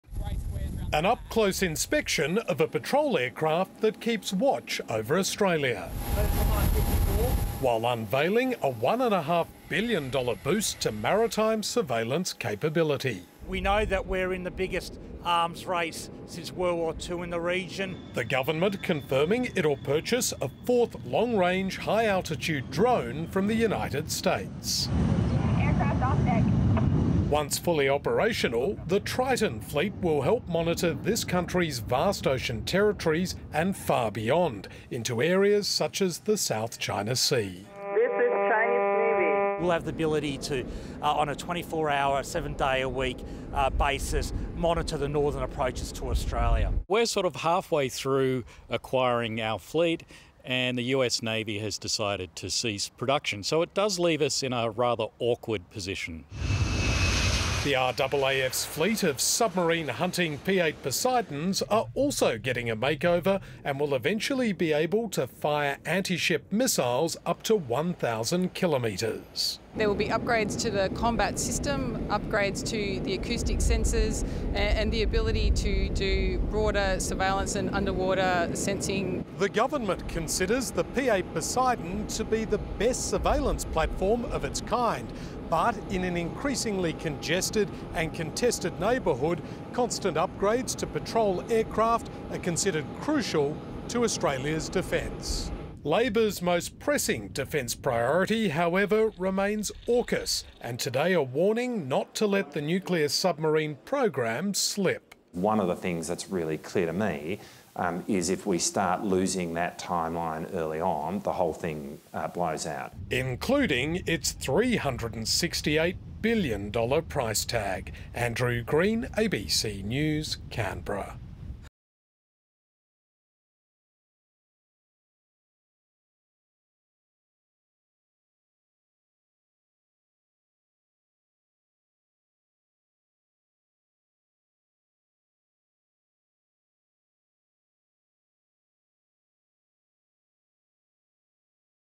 RAAF to get new aircraft and drones worth $1.5 billion | ABC News
The Air Force will get a $1.5 billion boost for maritime surveillance aircraft to better protect the country's northern approaches.
The package includes the purchase of a fourth long-range Triton drone, despite the US Navy ending its production soon.